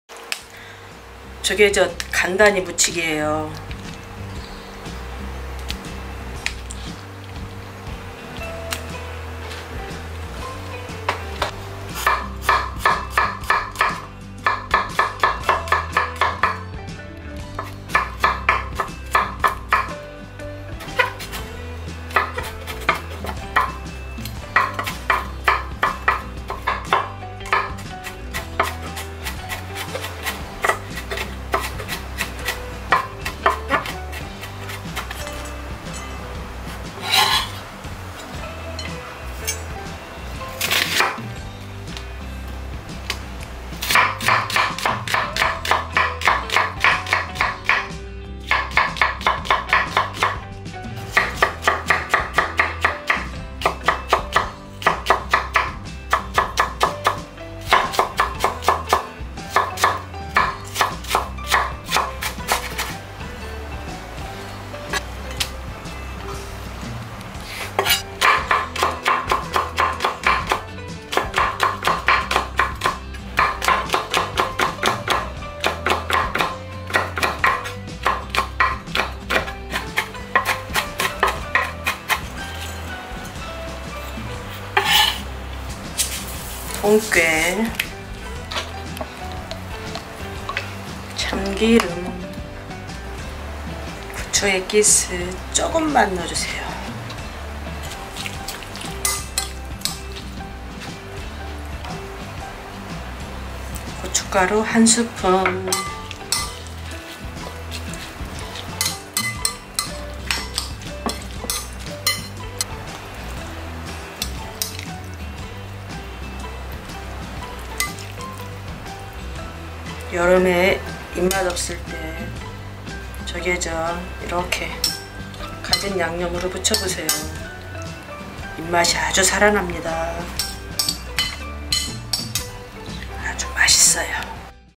찜통으로 잃었던 입맛 !!후딱돌아오는 조개젓무침 / Salted clams spicy seasoning - Korean Homecook
Introducing Korean cuisine.